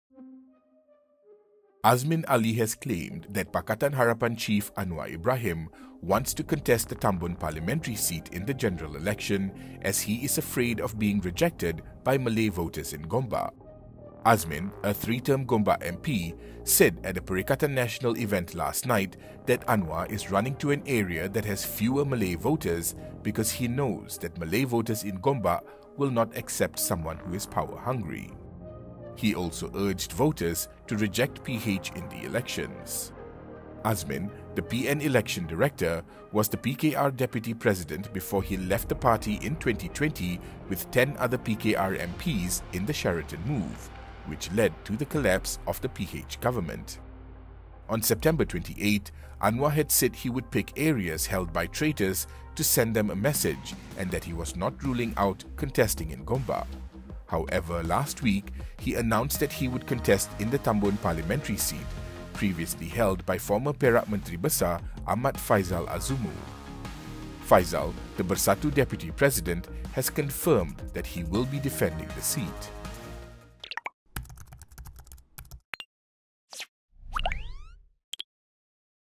Anwar ‘running' to Tambun as he fears rejection in Gombak, says Azmin
Former PKR deputy president Azmin Ali says Anwar Ibrahim is afraid of being rejected by Malay voters in Gombak.